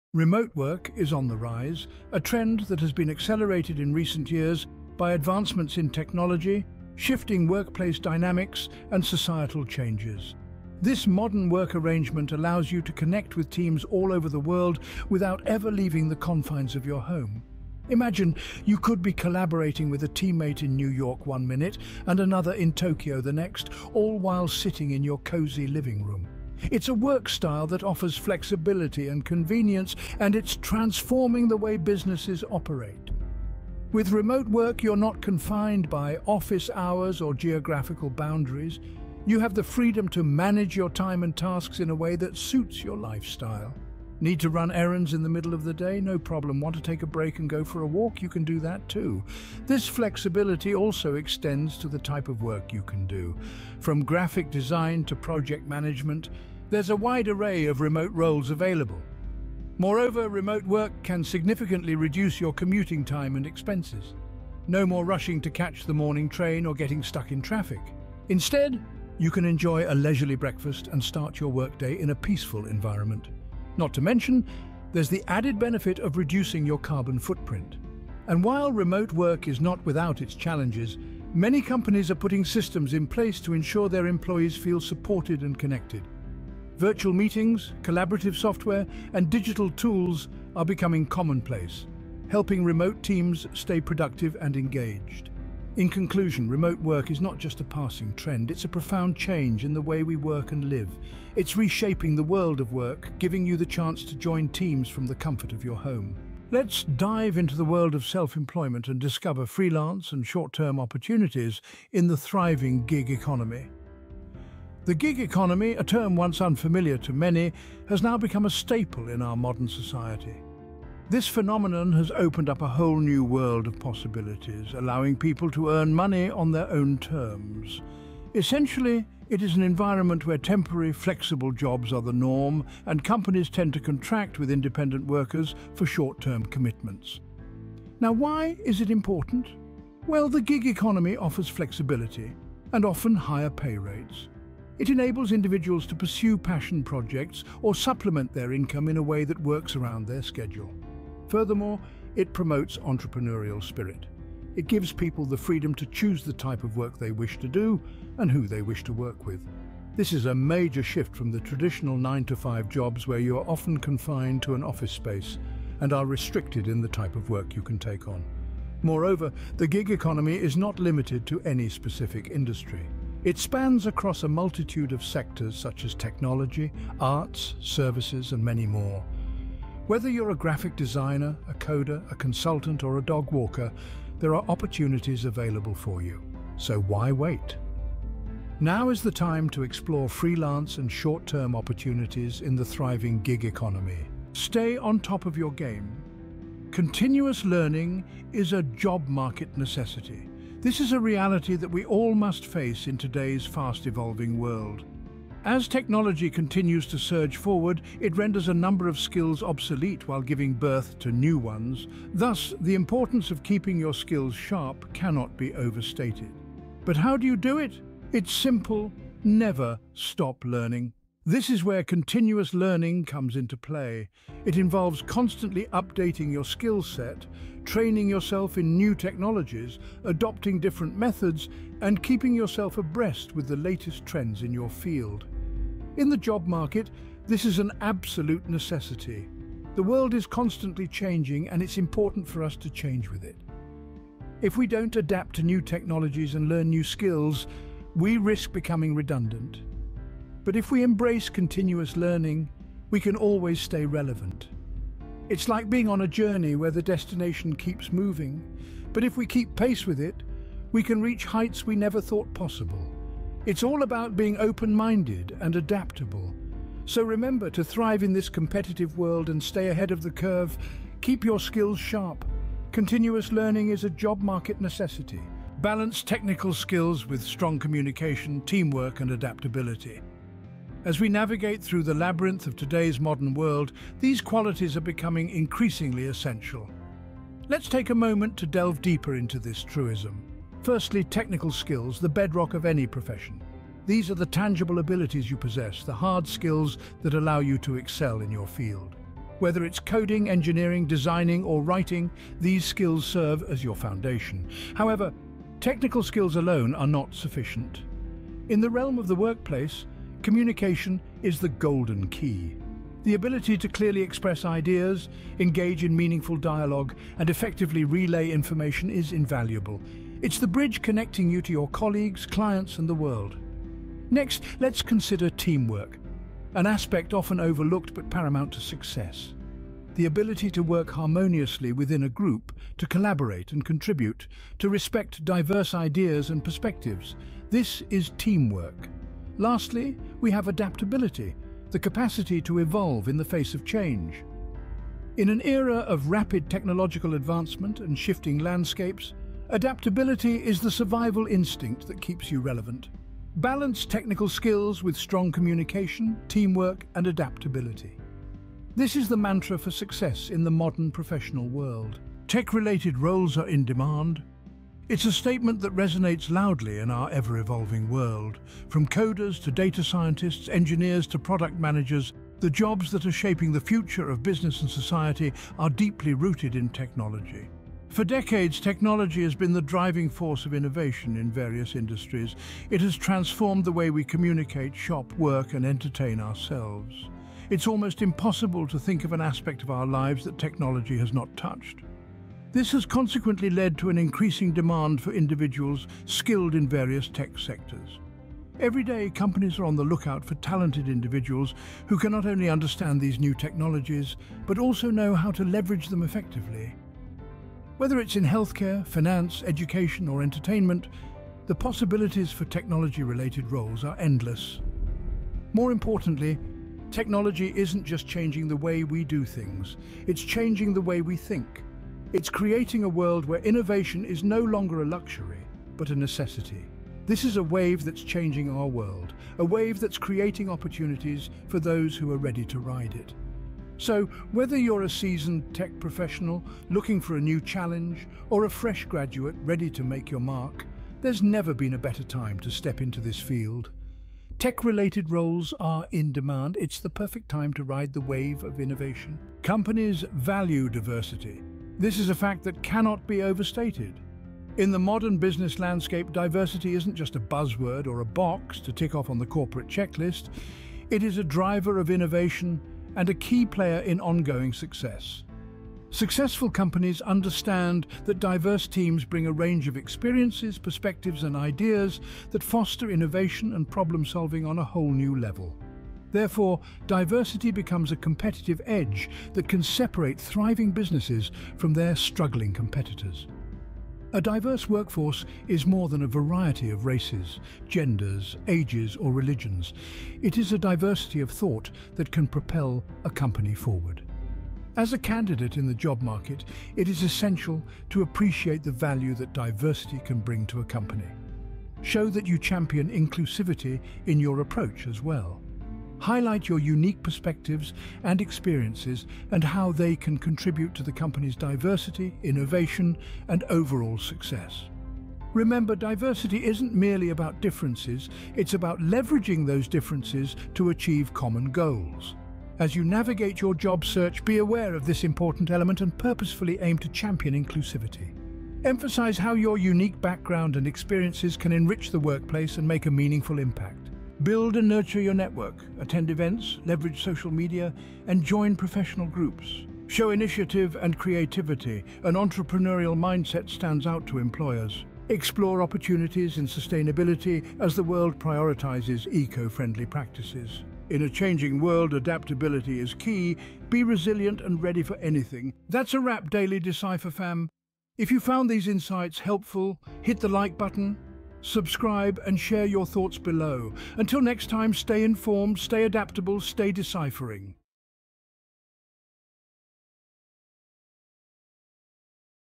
Navigating Job Opportunities Today: 10 Must-Know Facts || Daily Decipher
🚀 Dive into the dynamic world of job opportunities with Daily Decipher! 🌐 Uncover the latest trends shaping the job market and boost your career with our must-know facts! From the Remote Revolution to the Gig Economy Boom, we've got the insights to help you navigate the present landscape.
🌍 Topics Covered:
1️⃣ Remote Work Rising
2️⃣ Gig Economy Exploration
3️⃣ Non-Negotiable Upskilling
4️⃣ Spotlight on Soft Skills
5️⃣ Riding the Tech Wave
6️⃣ Embracing Diversity and Inclusion
7️⃣ The Timeless Power of Networking
8️⃣ Entrepreneurial Mindset Magic
9️⃣ Sustainability Careers Surge
🔟 Adaptability: Your Career Superpower
💡 Ready to elevate your career game? Stay informed, adaptable, and keep deciphering the secrets to success with Daily Decipher!